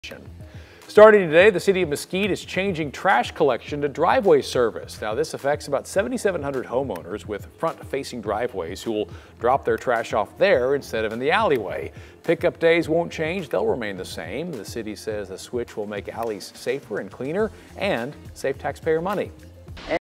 Mesquite, Texas beginning driveway trash service
Residents will have to bring their trash to their driveways instead of alleyways.